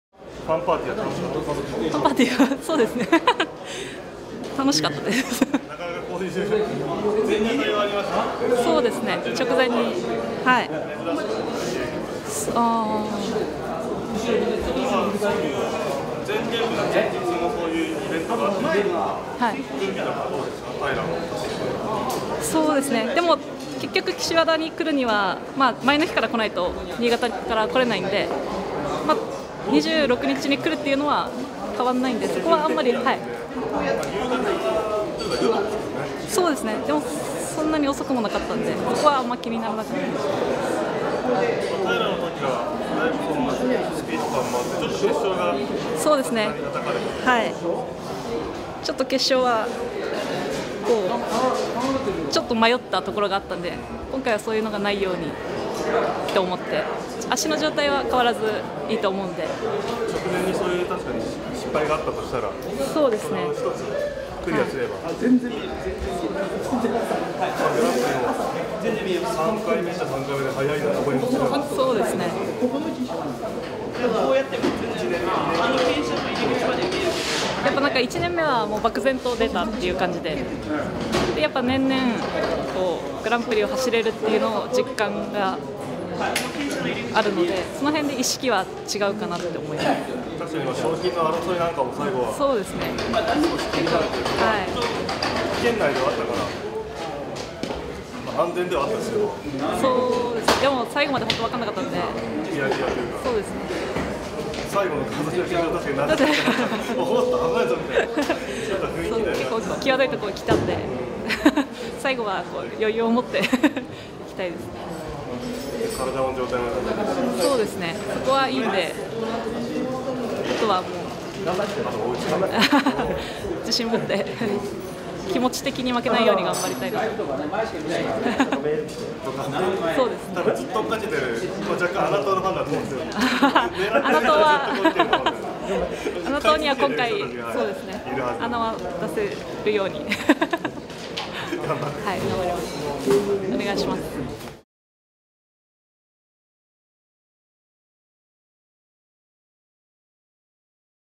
【ガールズGP2014】ほっこり笑顔の中川がGPでスマイル爆発させる！
ほっこり笑顔がトレードマークの中川諒子（３０）が、２８日に最高のスマイルを爆発させる。ガールズケイリンの開始から安定した成績を残してきた中川。大きなレースにも何度も出場しているが、結果は残せていない。今回こその思いを胸に秘め、集中力を高める。